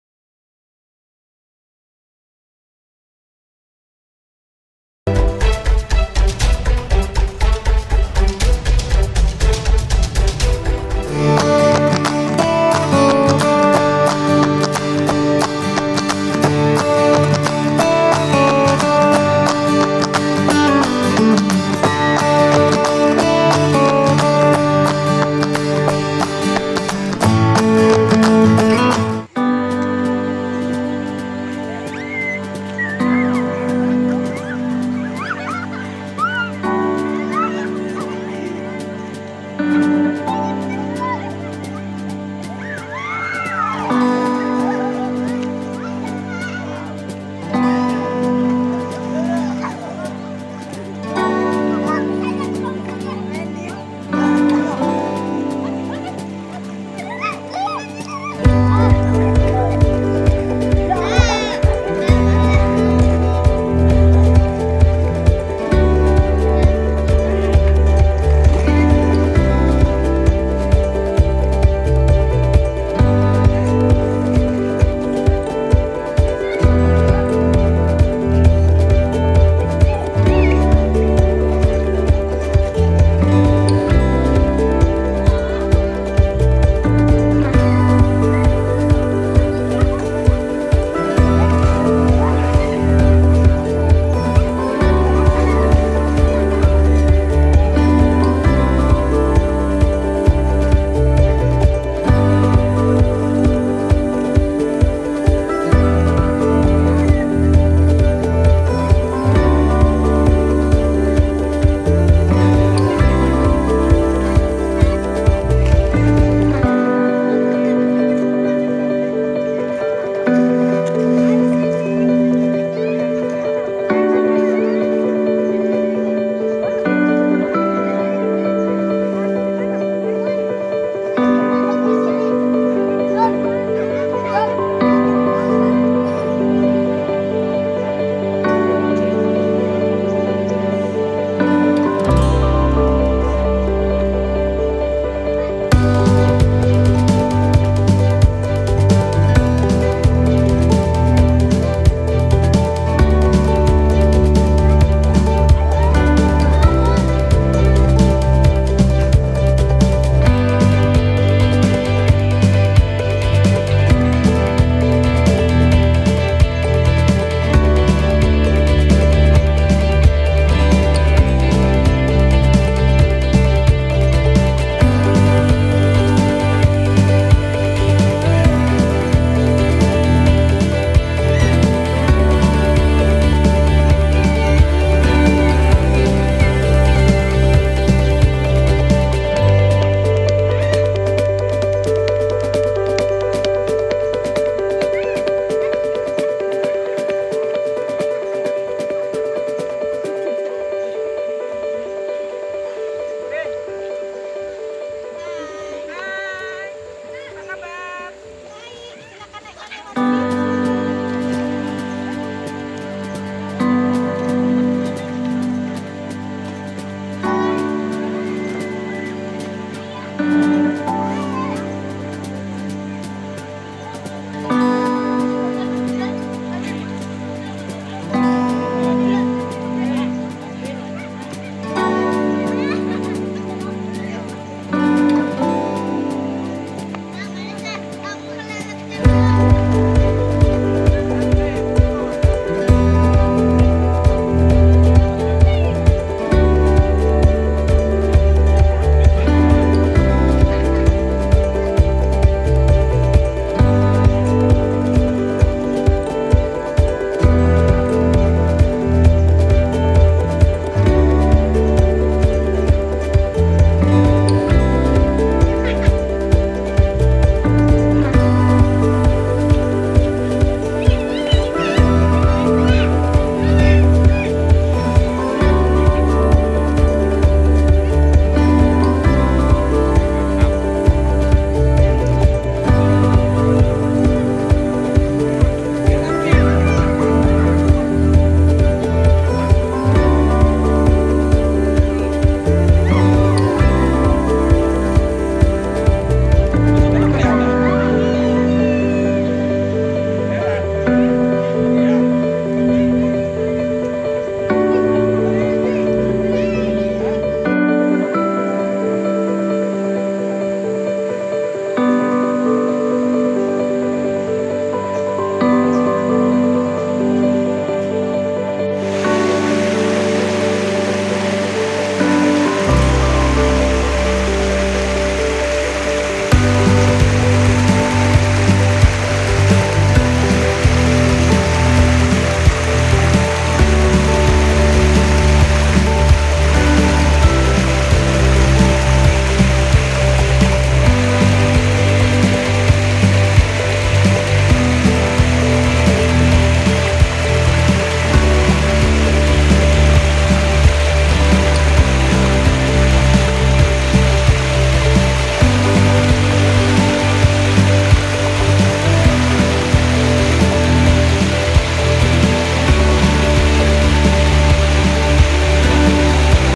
Holiday Wisata Riam Mahoni Binuang Kalsel
Riam Mahoni termasuk tempat wisata yang baru lagi viral terletak di Kecamatan Binuang Kabupaten Tapin, Kalimantan Selatan.
Pengunjung yang datang ke wisata ini untuk menikmati keindahan alam, kolam renang, water park dan relief serta air terjun.
Rombongan kami satu RT berlibur ke tempat wisata ini karena masih baru dan membina silaturahmi.